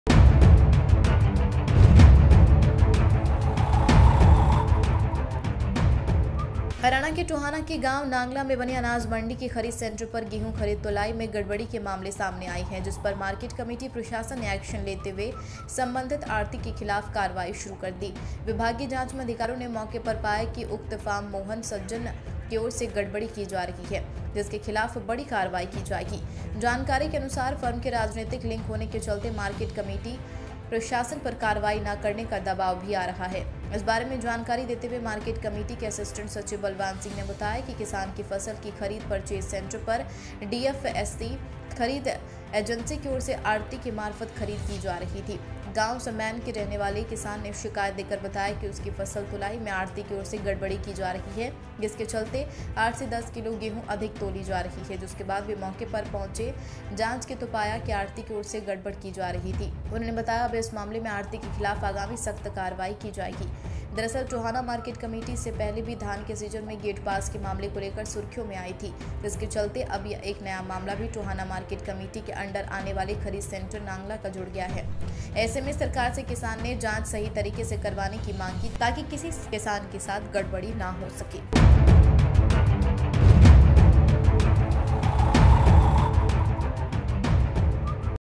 HARYANA NEWS: गेंहू खरीद में गड़बड़ी का Video वायरल...
हरियाणा के टोहाना के गांव नांगला में बनी अनाज मंडी के खरीद सेंटर पर गेंहू खरीद तुलाई में गड़बड़ी के मामले सामने आया हैं. जिसपर मार्केट कमेटी प्रशासन ने एक्शन लेते हुए संबंधित आढ़ती के खिलाफ कार्रवाई शुरू कर दी है. विभागीय जांच में अधिकारियों ने मौके पर पाया कि उक्त फर्म मोहन सज्जन की ओर से गड़बड़ी की जा रही है. जिसके खिलाफ कड़ी कार्रवाई की जाएगीं जानकारी के अनुसार फर्म के राजनीतिक लिंक होने के चलते मार्किट कमेटी प्रसाशन पर कार्रवाई न करने का दबाव भी आ रहा है.